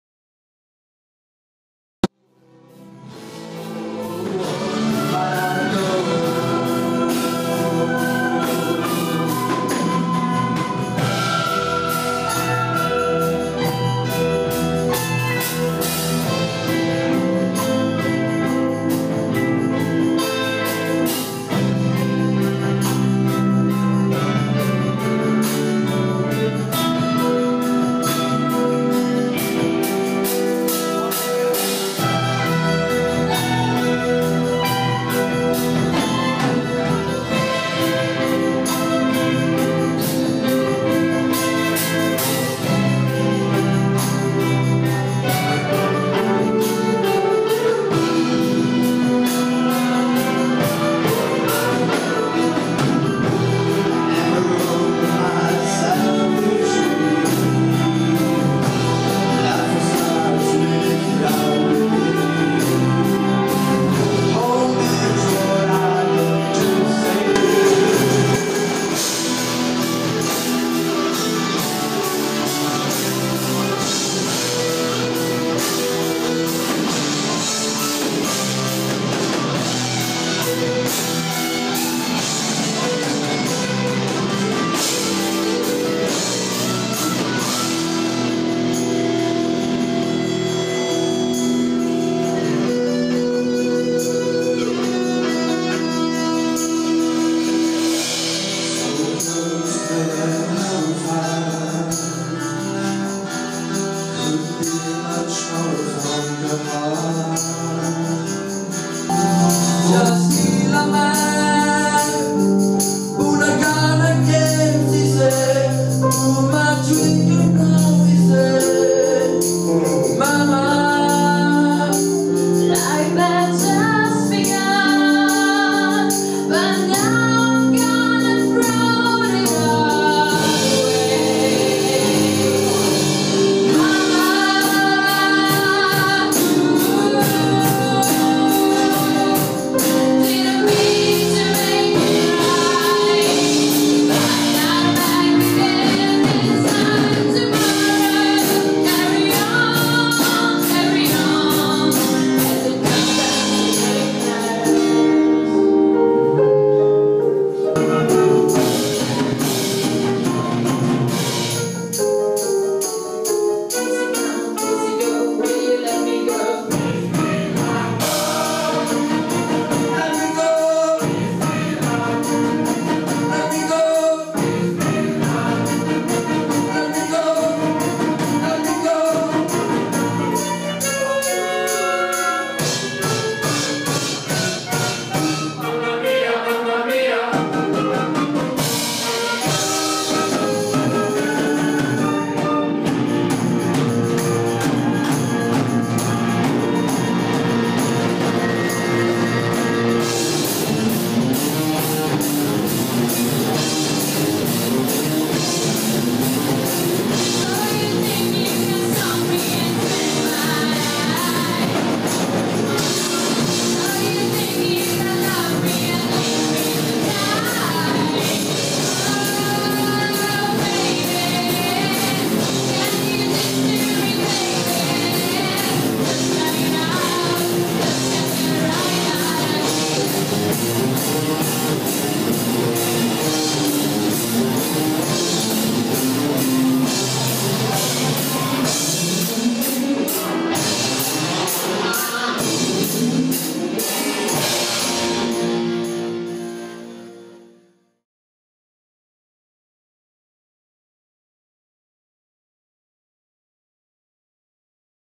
Calini Ensemble - Prove 2013
Calini Ensemble è l'orchestra del Liceo Calini di Brescia che da anni ripropone classici del repertorio pop rock e brani originali. La direzione dell'ensemble è affidata ai musicisti Nicola Panteghini e Paolo Buzzi, in collaborazione con la prof. rita Lorenzini e con l'Associazione Musiclub.